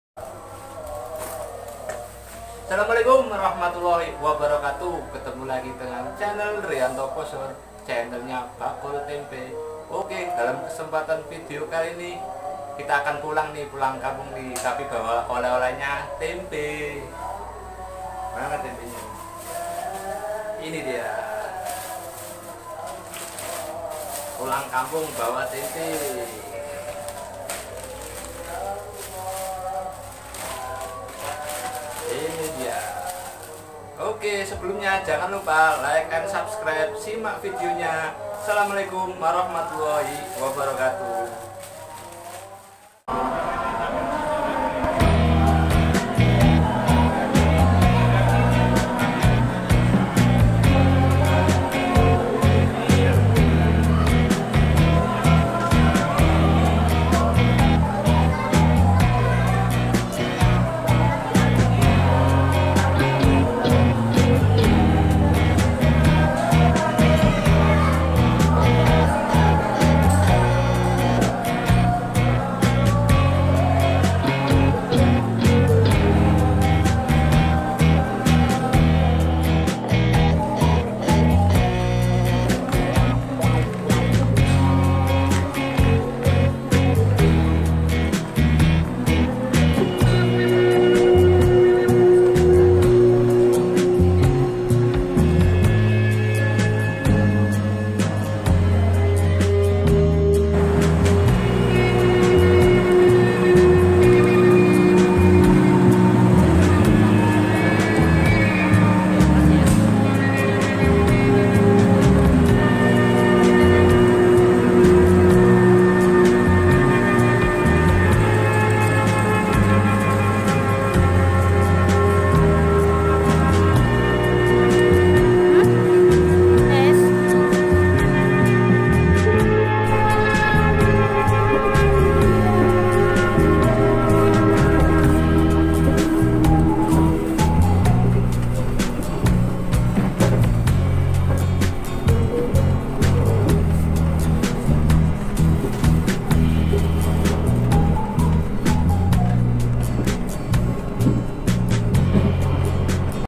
cara bakul tempe mudik
cara bakul tempe mudik,,,dg mem bawa oleh-oleh tempe,,,ini sebagai simbol aja,,, karena saya pedagang tempe,,,jadi mungkin lebih spesial bawa tempe kekampung,,,bisa dikasihkan orang tua atau saudara,,,bukan tempenya yg spesial tapi nilainya itu yg menurut saya lebih unik gitu...